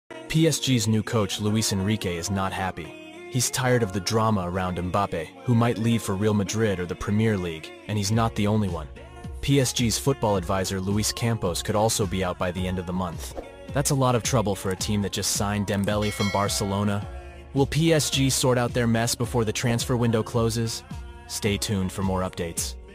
Luis Enrique and Luis Campos Unhappy with PSG's Mbappe situation.
PSG's new coach Luis Enrique is not happy. He's tired of the drama around Mbappe, who might leave for Real Madrid or the Premier League.

And he's not the only one. PSG's football advisor Luis Campos could also be out by the end of the month.

That's a lot of trouble for a team that just signed Dembele from Barcelona.

Will PSG sort out their mess before the transfer window closes?